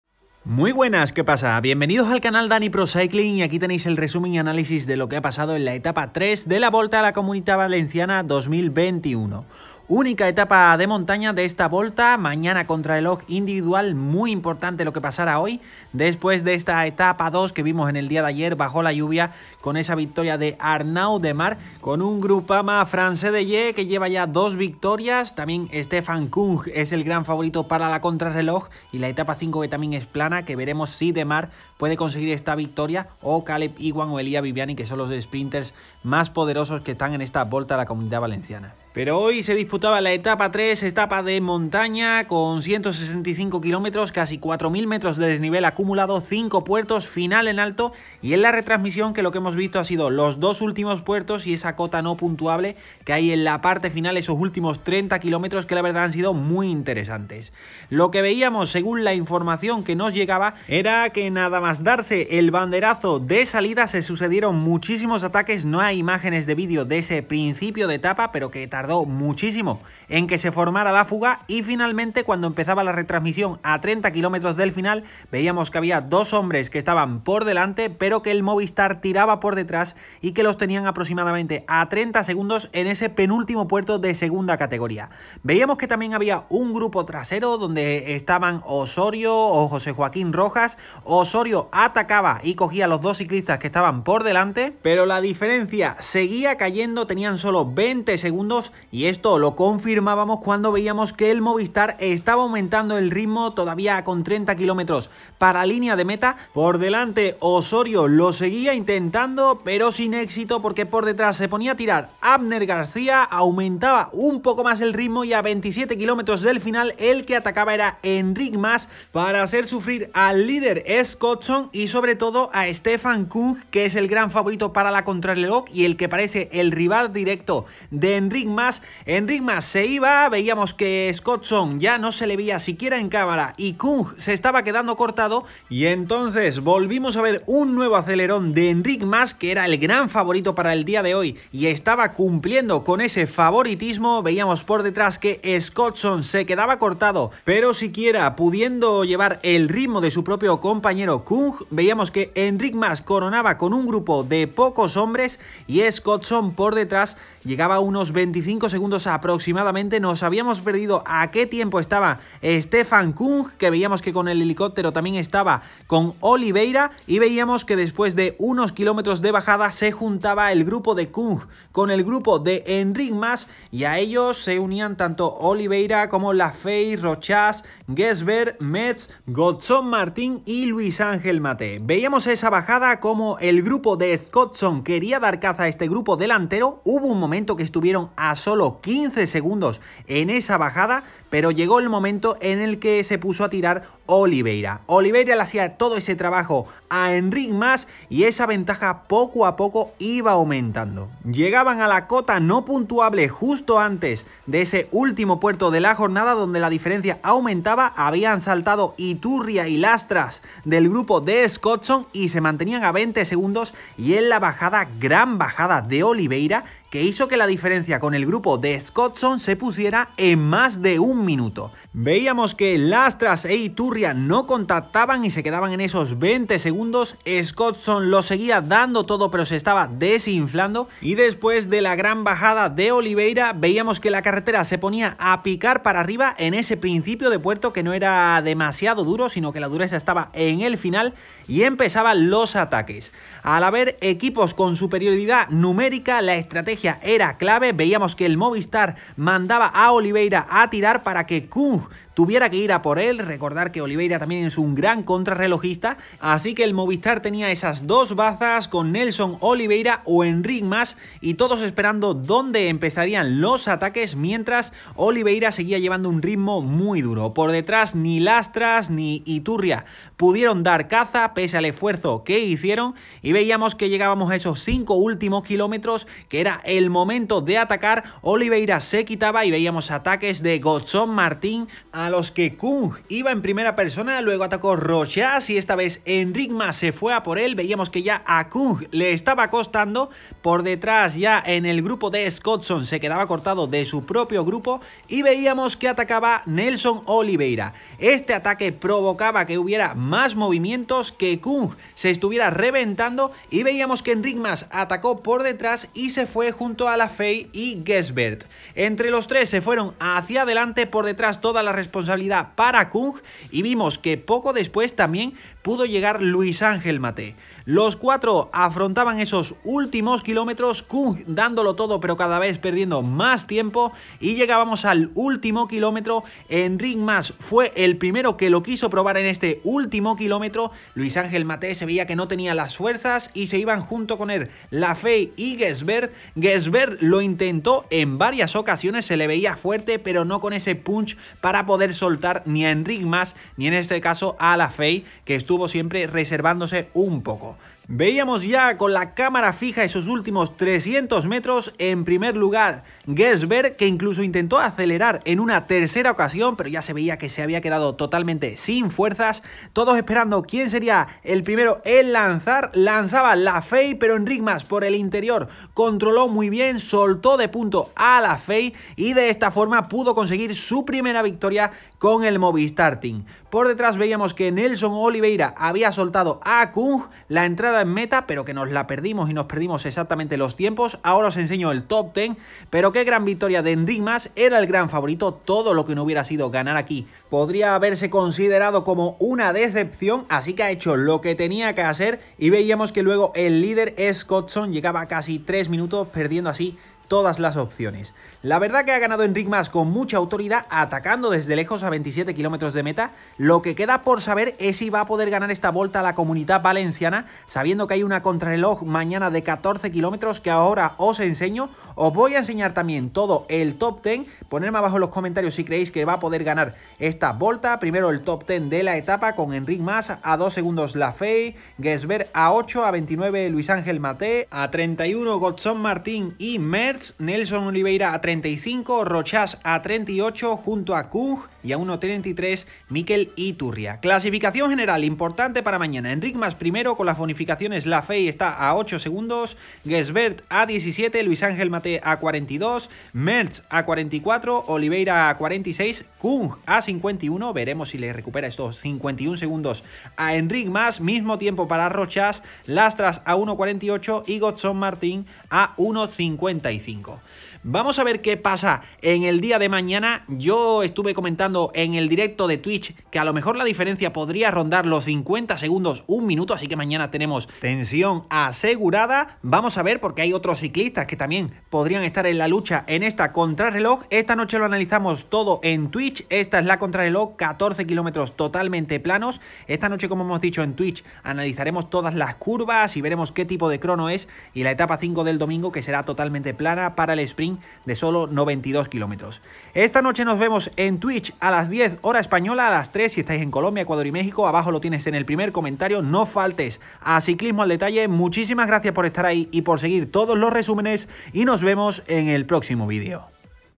RESUMEN ETAPA 3 ➤ Volta C. VALENCIANA 2021 🇪🇸 Enric MAS se Estrena
Se ha celebrado la etapa 3 de la Volta a la Comunitat Valenciana 2021 o Vuelta a la Comunidad Valenciana 2021. Enric Mas ha ganado la única etapa de montaña y se coloca como líder de la clasificación general. Primero atacó a 27 kilómetros del final para soltar Scotson y en el último puerto soltó a Kung, que parece el gran rival para la contrarreloj. Os dejo la clasificación completa. Todo en los highlight del resumen de Dany Pro Cycling de esta competición de ciclismo internacional.